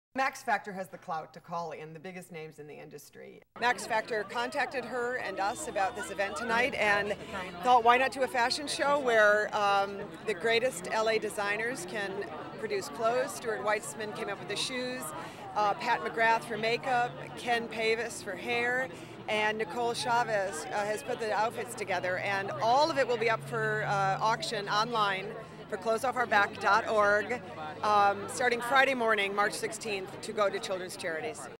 Celebrity Gossip by Jane Kaczmarek of "Malcom in the Middle" at Celebrity Charity Event
"Malcolm in the Middle" actress Jane Kaczmarek during her celebrity interview on the red carpet at the Max Factor Salutes Hollywood Fashion talks about the "Clothes on Our Back" charity and about the different people involved in running the fashion show.  For more celebrity news, gossip, and red carpet interviews check out more from SnapSnap.